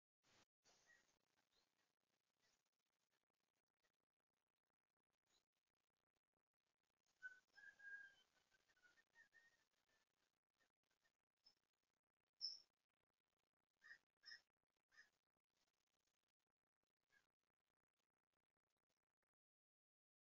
A curious deer checking out our veggie garden (captured by our security camera)
I guess it didn’t like our overgrown lettuce… 😆